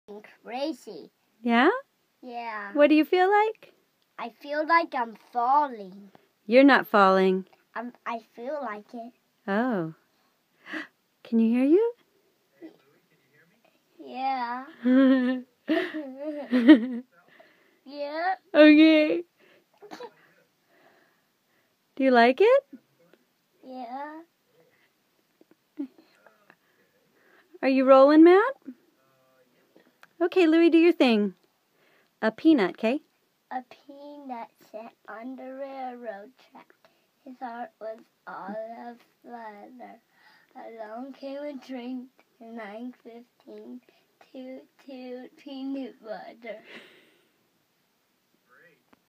Peanut in the Studio
Three year old Louis makes his debut in the recording studio. Marigny Recording Studio, New Orleans, Jan. 17. 2015.